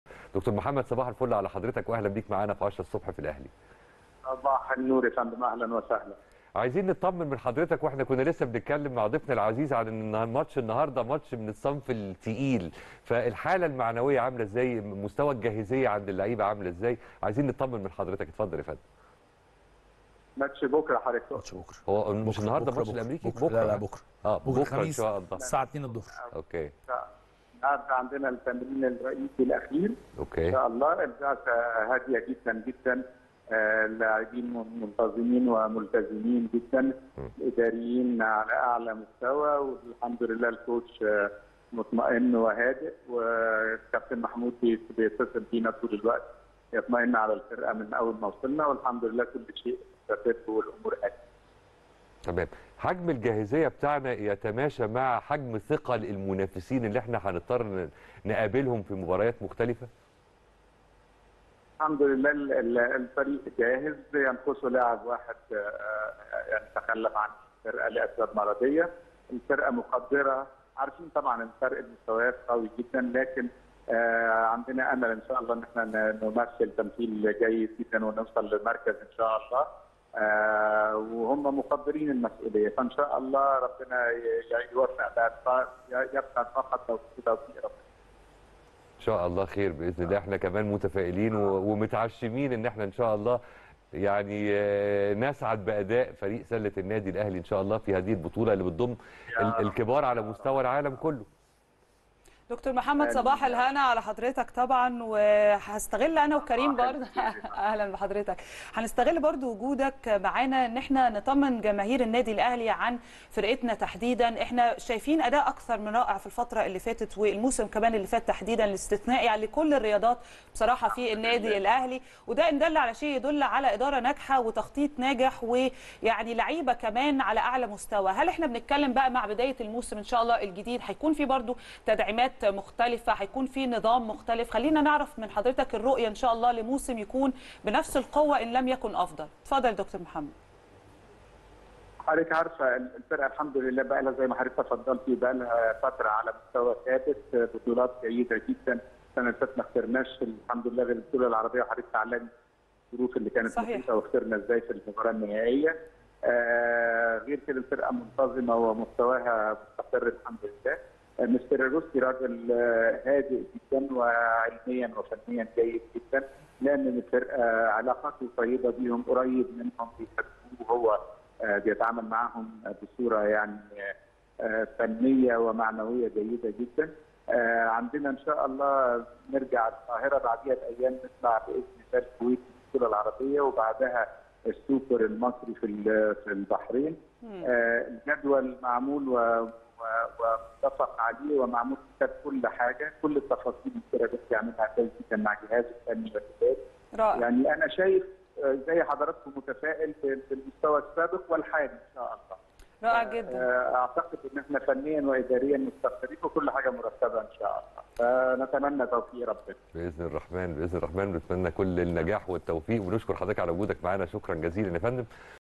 د. محمد شوقي رئيس بعثة الأهلي في سنغافورة: المعنويات مرتفعه وجاهزون لضربة البداية في مونديال السلة
د. محمد شوقي عضو مجلس الإدارة ورئيس بعثة الأهلي في سنغافورة: المعنويات مرتفعه وجاهزون لضربة البداية في مونديال السلة غدا.